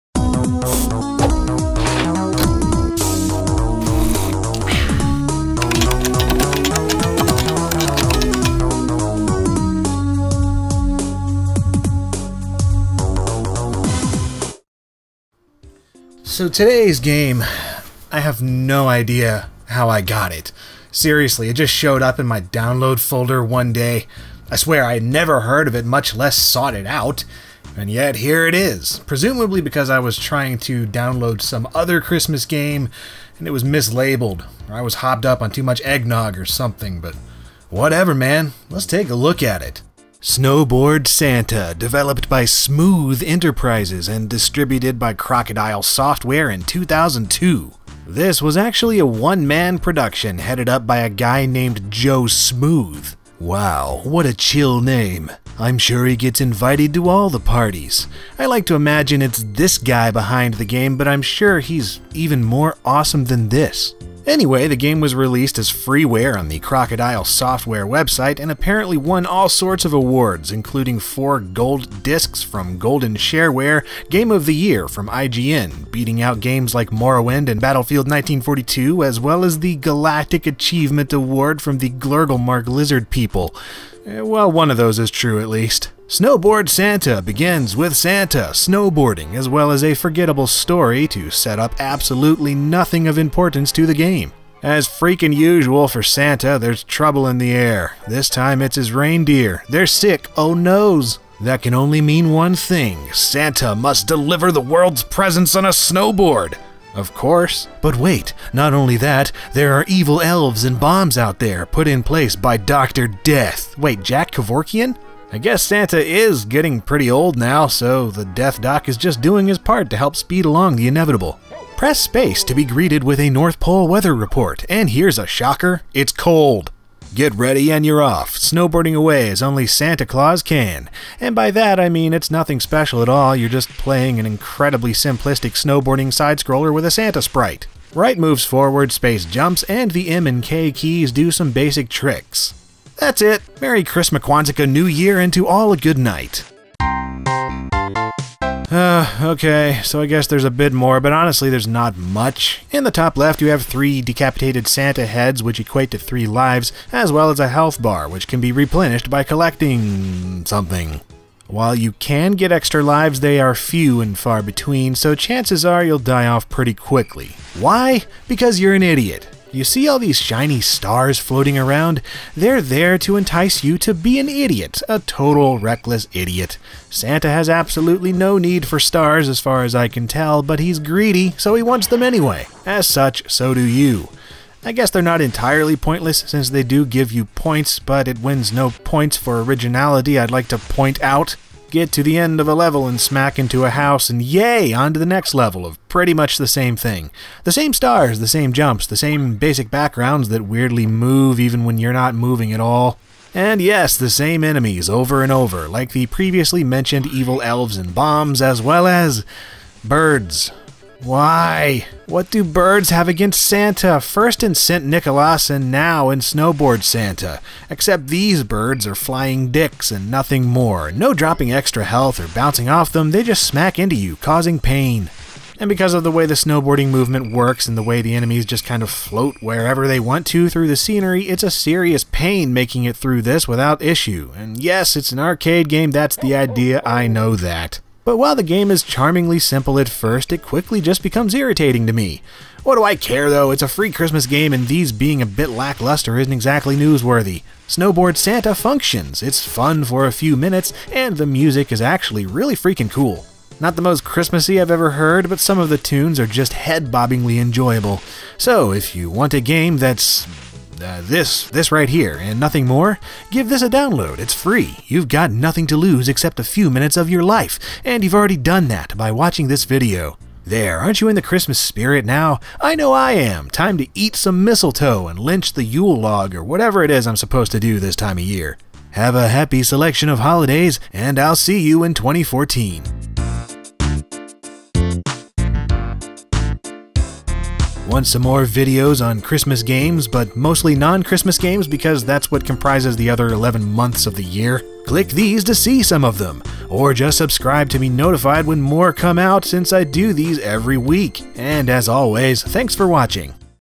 LGR - Snowboard Santa - PC Game Review
The reindeer are sick, what is Santa to do? Deliver the presents by snowboard, naturally!